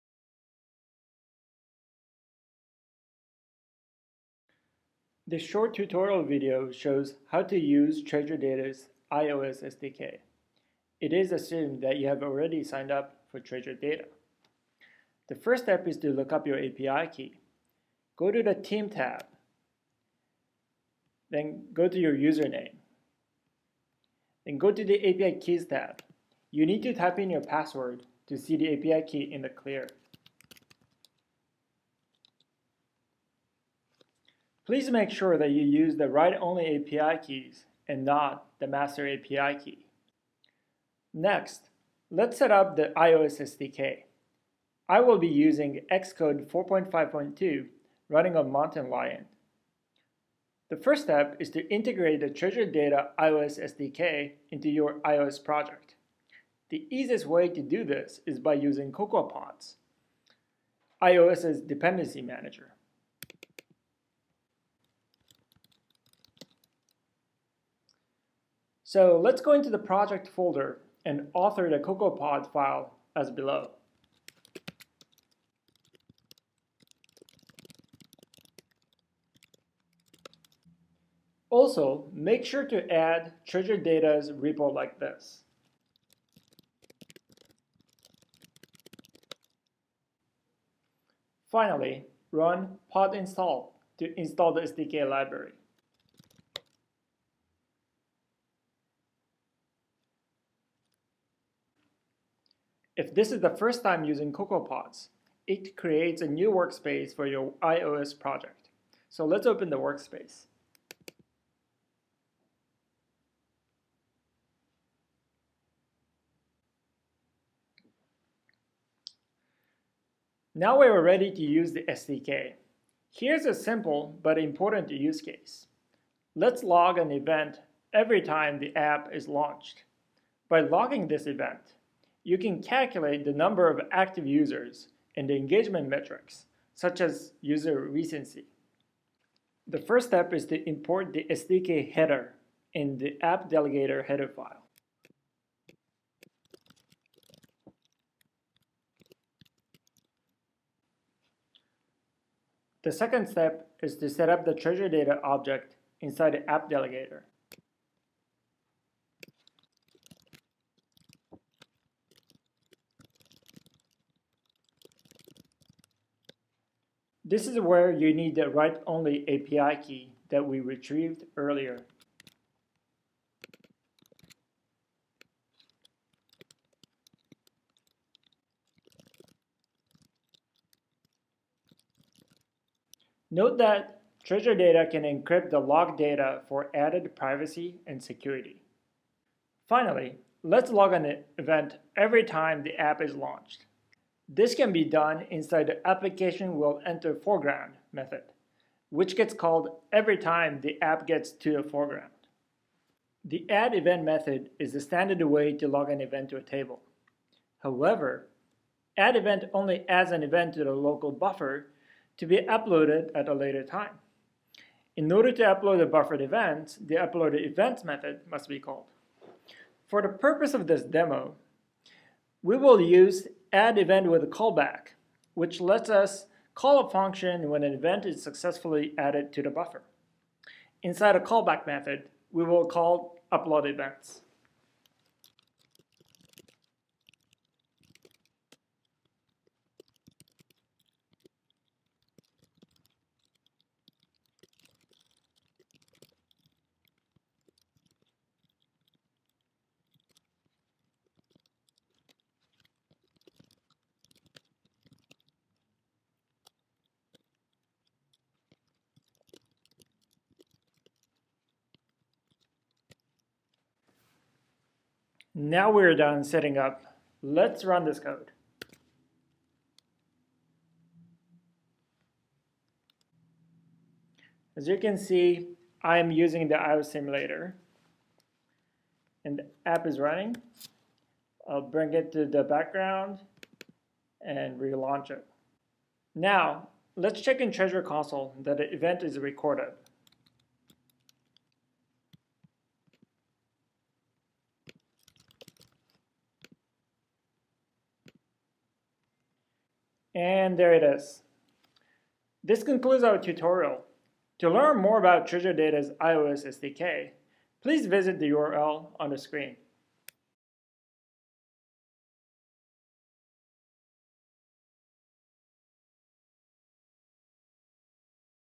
Install iOS SDK for Real-Time Data Collection - Treasure Data
This short tutorial video shows how to use Treasure Data's iOS SDK to collect streaming data from iOS applications into Treasure Data's cloud service.